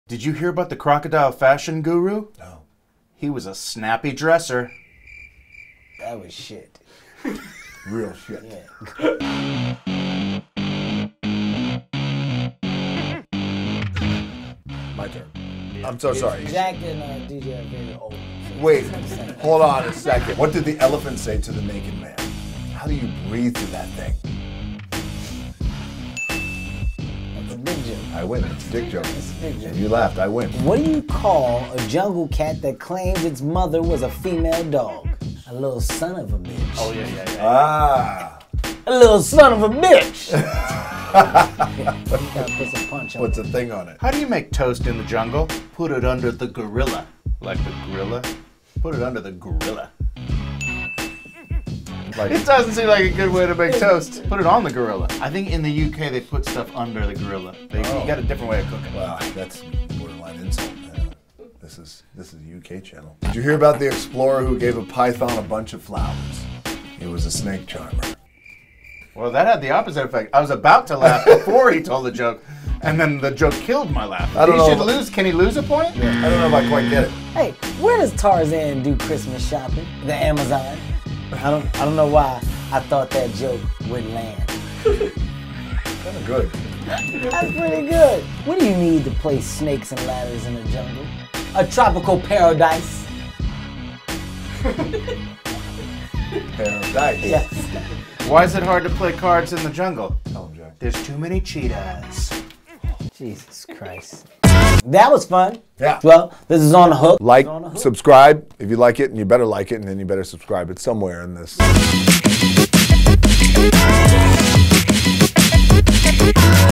The Rock, Kevin Hart & Jack Black Tell The Worst Jokes Ever Made
Jumanji stars The Rock a.k.a Dwayne Johnson, Kevin Hart and Jack Black try to make each other laugh with some of the worst jokes you'll ever hear.